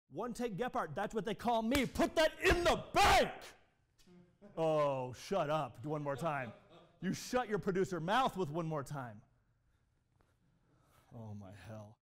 IN THE BANK SON!
When you NAIL a complicated stand-up in one take but the producer gets in your ear and tells your to "do it again" this time "snappier."